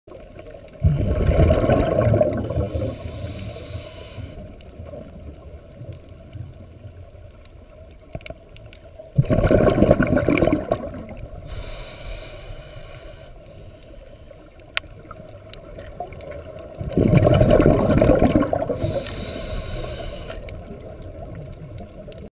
Scuba Diving Swim Through #2
Another "swim through" we experienced diving off the coast of Key Largo, Florida